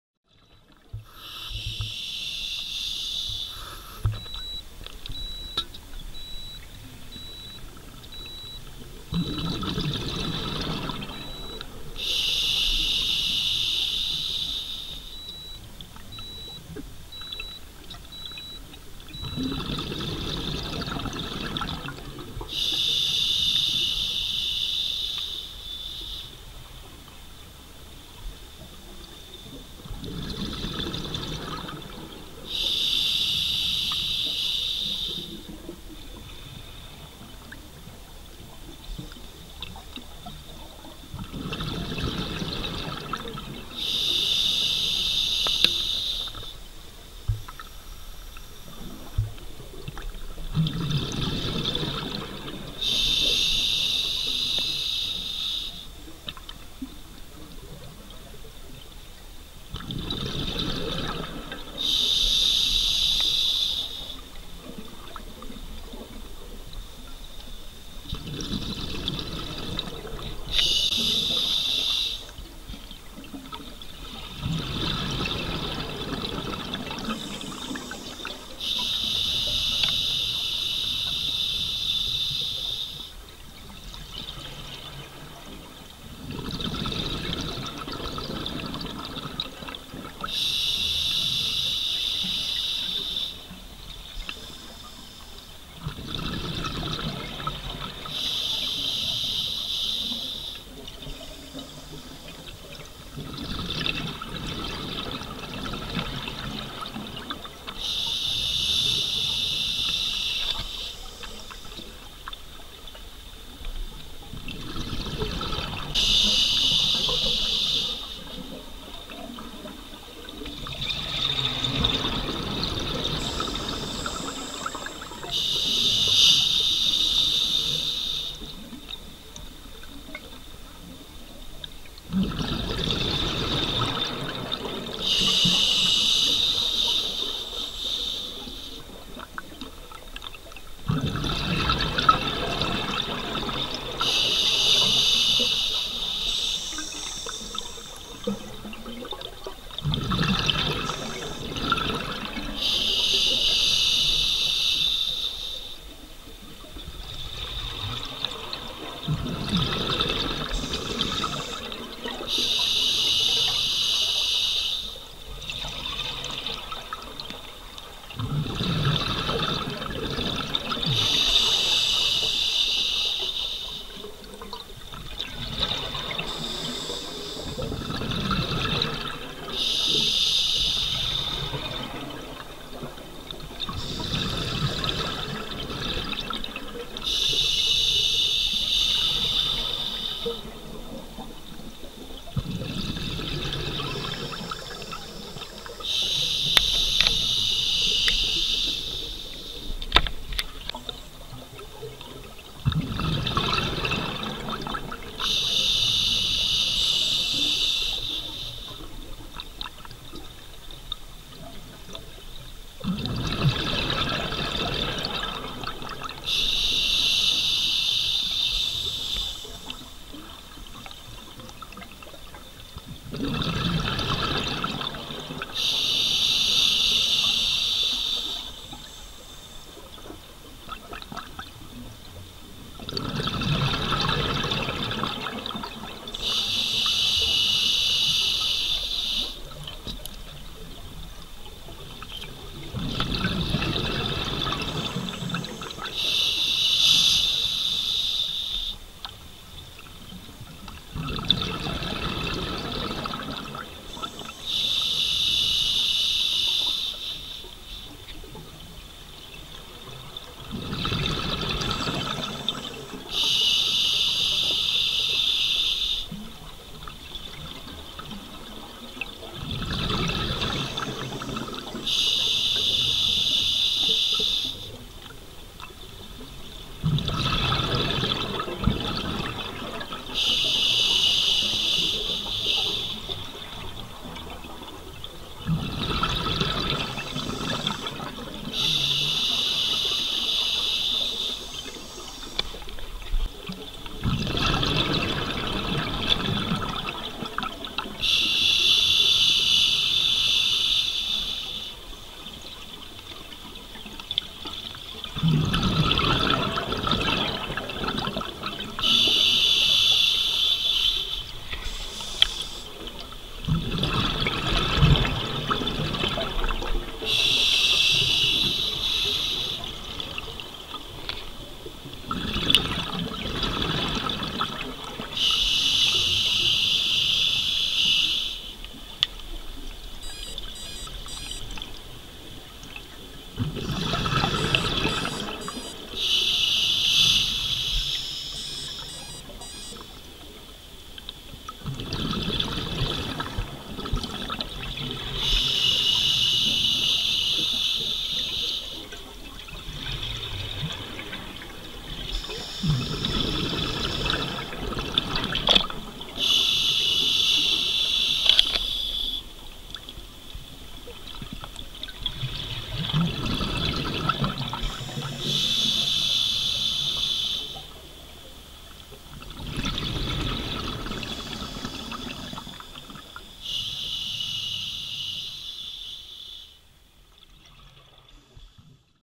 Tauchgang Walensee CH, Ladiwracks auf 34m Tiefe Juli 2010
Tauchgang im Walensee bei Walenstadt in der Schweiz.
Die Ladiwracks liegen auf ca. 34m Tiefe und sind direkt vom Ufer aus erreichbar.
gefilmt 5.7.2010